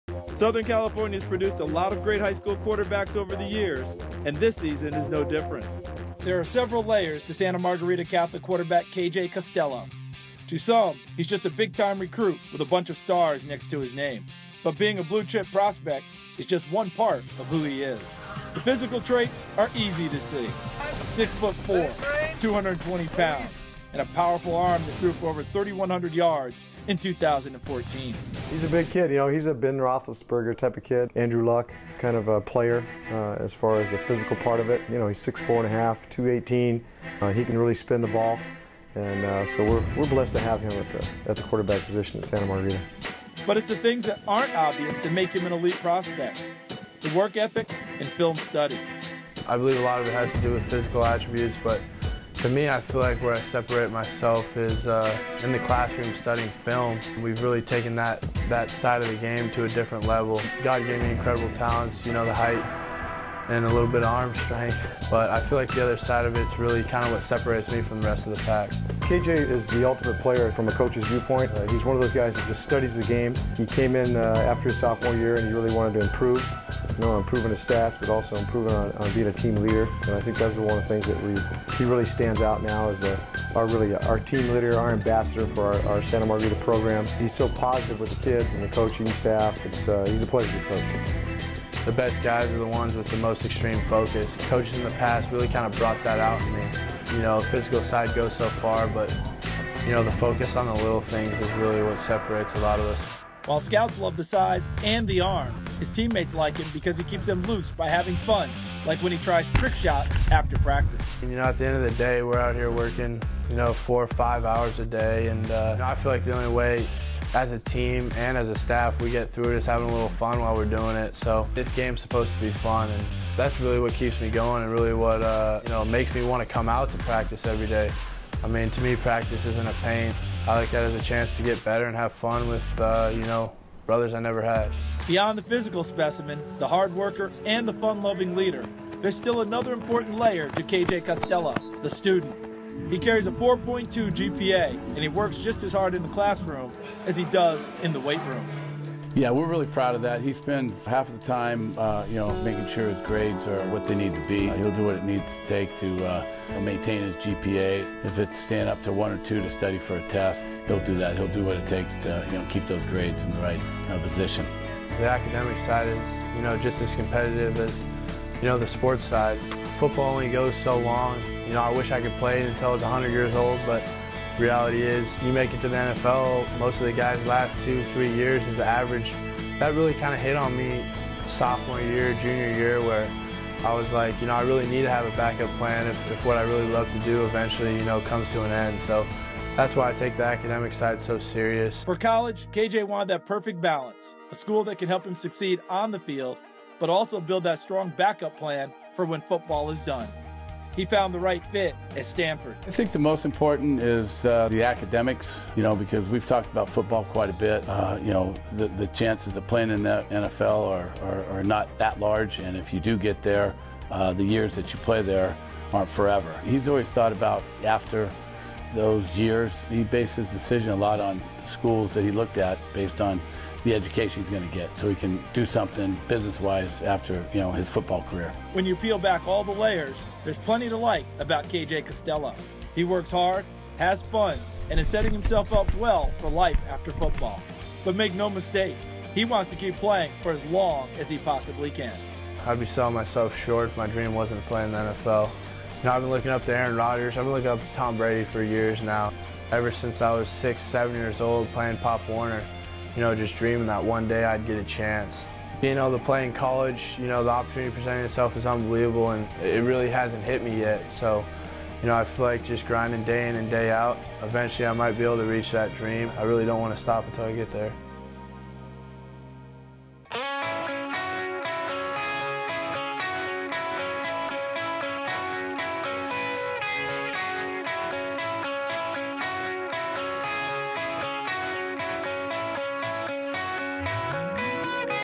KJ Costello - Santa Margarita Catholic - Highlights/Interviews - Sports Stars of Tomorrow
K.J. Costello is the latest blue chip quarterback prospect to come out of Southern California. He's been a huge success on the field and in the classroom at Santa Margarita Catholic. The strong-armed QB is on his way to Stanford.